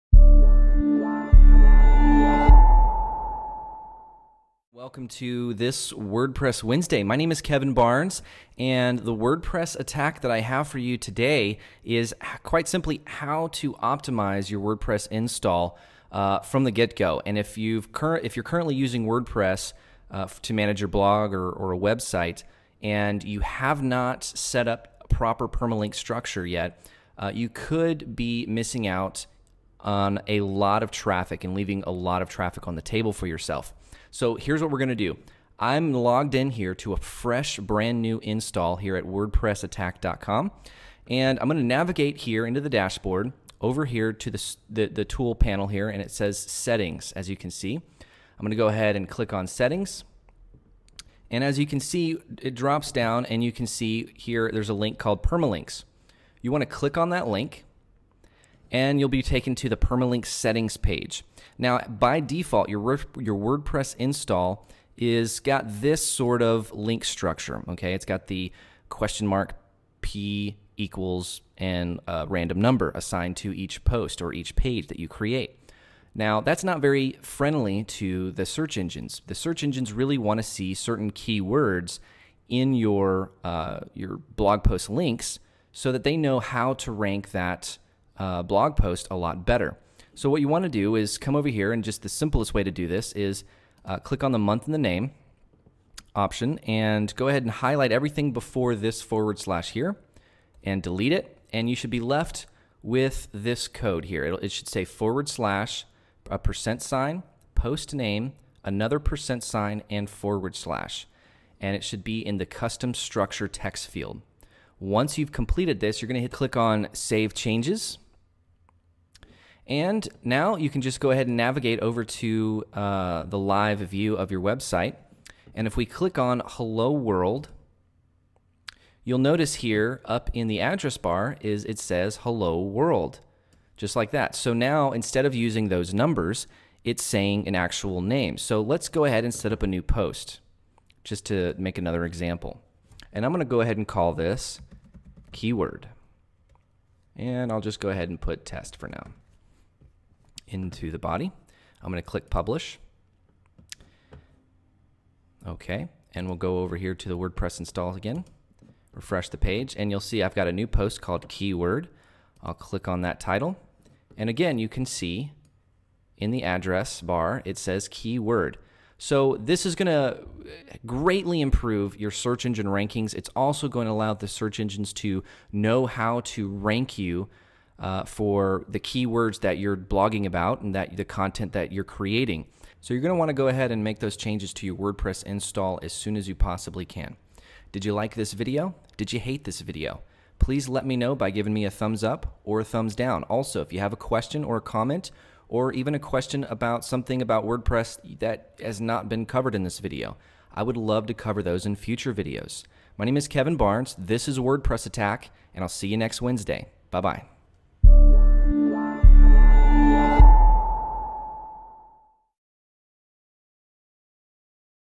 WordPress Permalinks - How To Do Them - WordPress Attack!
WordPress Permalinks are a bit confusing for some.  Kevin from WordPress Attack makes sense of optimizing your WordPress Site with the best possible permalinks. Catch more of Kevin, and other great videos